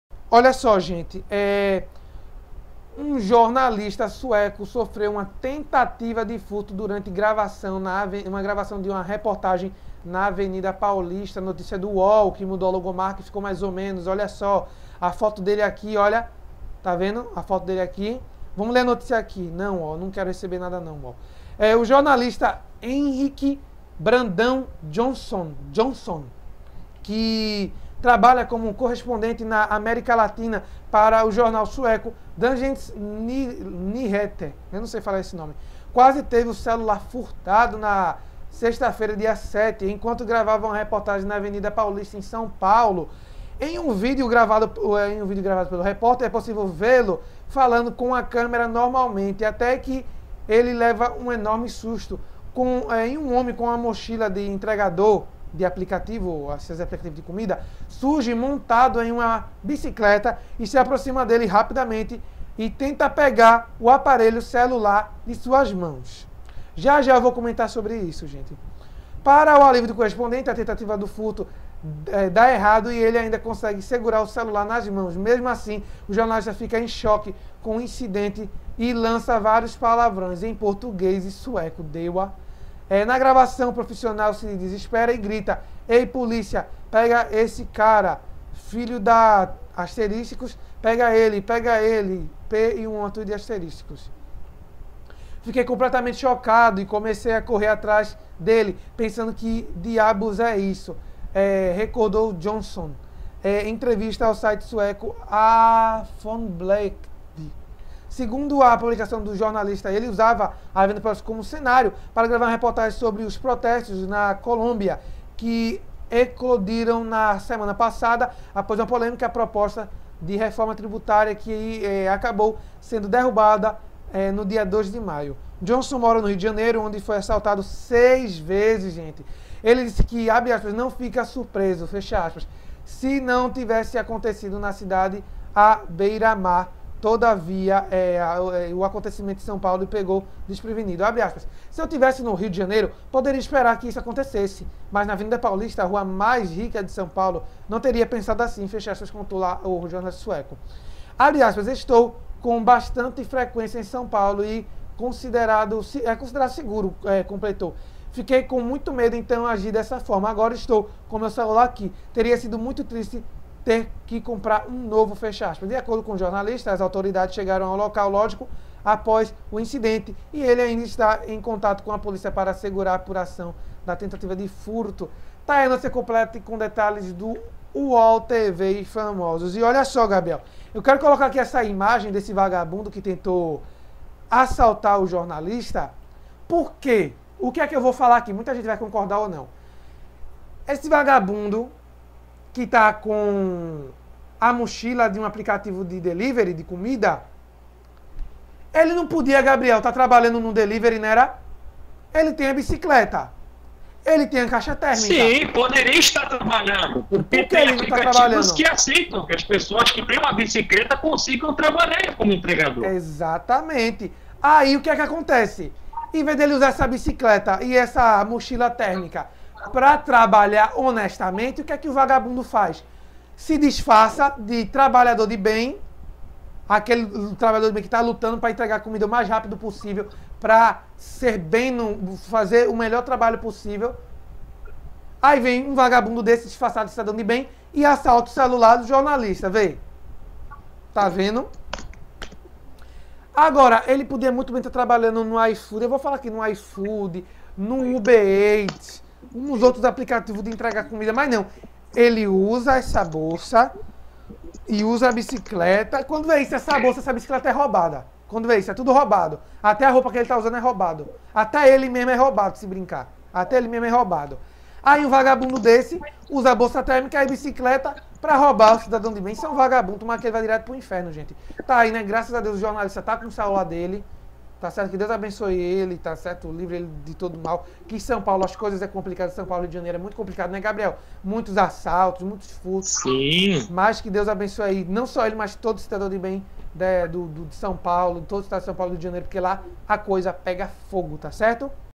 Jornalista sueco sofre tentativa de furto durante gravação na Av Paulista
Fala Mais - Interação, Informação, opinião e credibilidade é aqui.